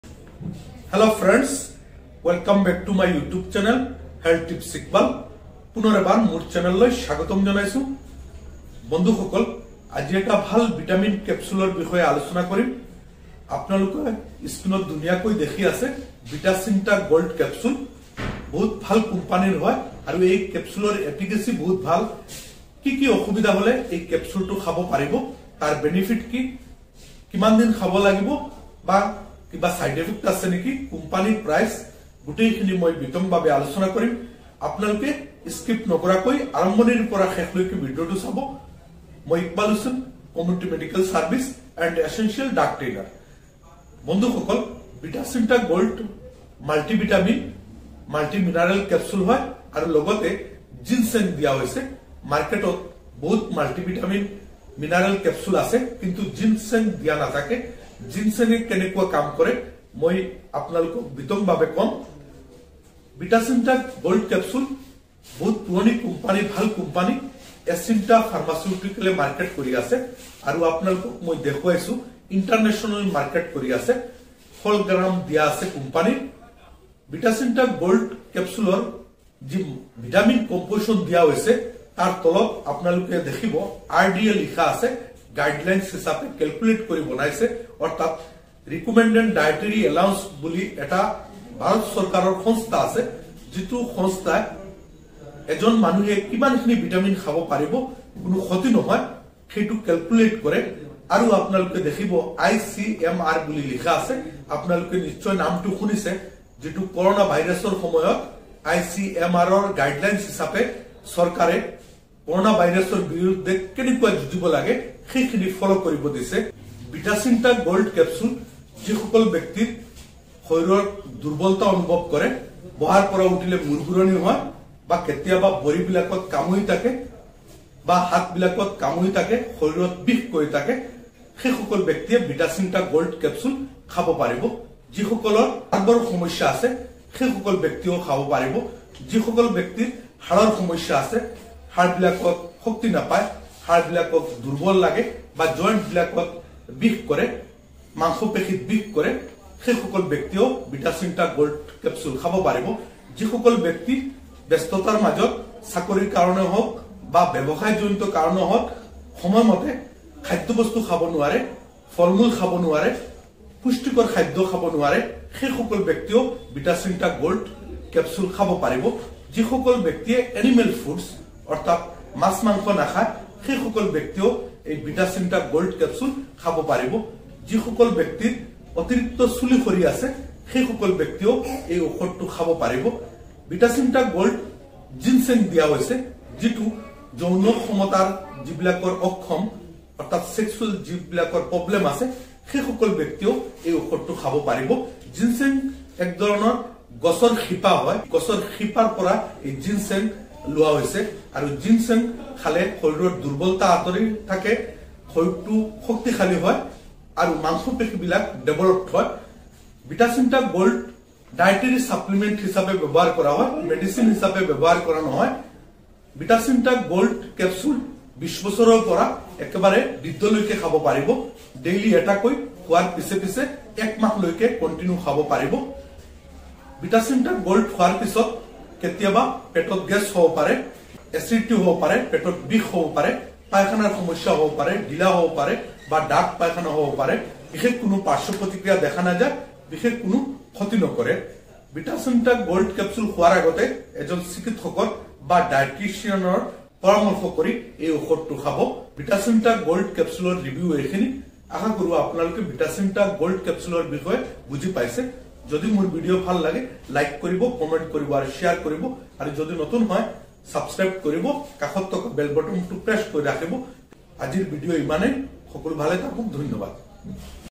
vitacinta GOLD capsule || vitacinta GOLD capsule use in Assamese || multivitamin ||health tips ikbal
vitacinta GOLD capsule//vitacinta gold capsule use, benefits,dose in Assamese//health tips ikbal

vitacinta GOLD capsule কি কি অসুবিধা হলে খাব পাৰিব, খোৱাৰ নিয়ম, কিমান দিন খাব পাৰিব সম্পূৰ্ণ জানি লওঁক অসমীয়া ভাষাত। health tips ikbal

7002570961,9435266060